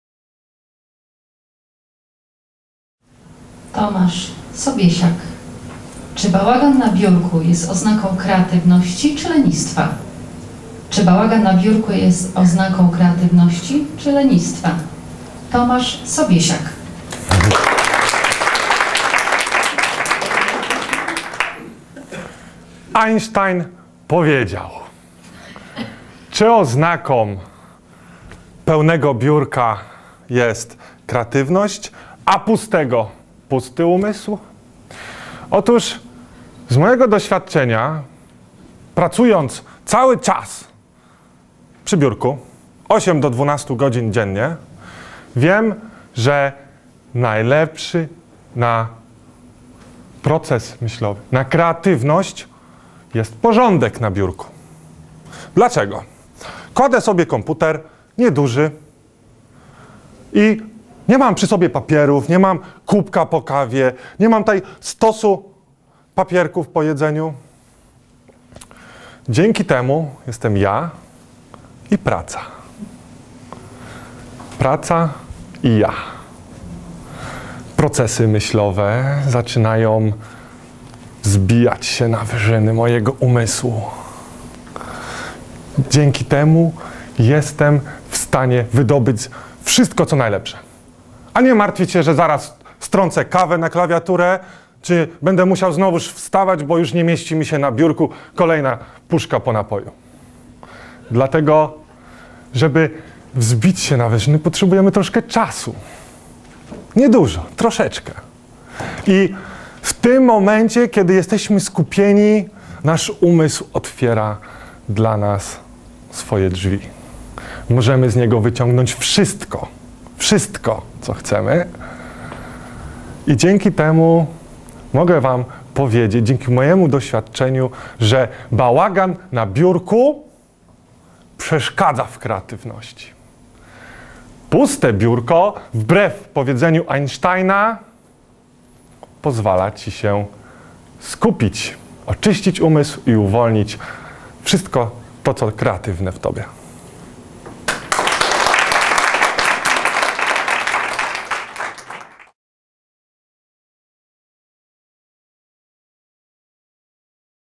Table Topics answer of Tomasz Sobiesiak
Contest speech for the Toastmasters District 59, Area M5 contest.

Filmed by: Krzysztof Nepelski, Pierwszy Krakowski Polskojęzyczny Klub Toastmasters.